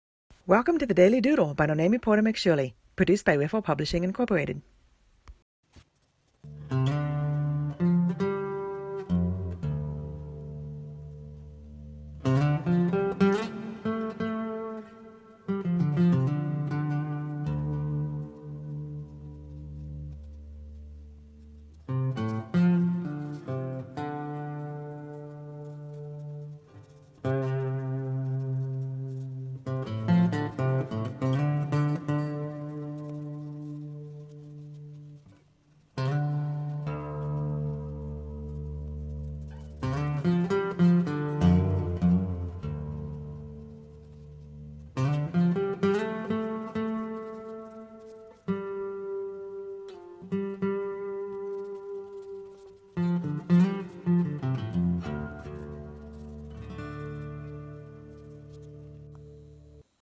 DAILY DOODLE: Live Oak Tree Trunk
Live Oak, also known as Virginia Live Oak, grows mostly along the Southeastern coast line of the United States, skimming the edges of half a dozen states, plus a good chunk of Texas and all of Florida. It’s called “Live Oak” because it is one of the few evergreen oaks, meaning it holds its small grayish-green leaves all year long. These trees grow to be so massive, that the  leafy branches would be mostly out beyond the edges of this sketch. “Every time I look at one of these sprawling tangles of trunks, which often bear a good deal of moss, I think it looks like there should be a bunch of monkeys in them. …I also think these trees look like endless fun for climbing on, and for sitting in with a good book.” --Noname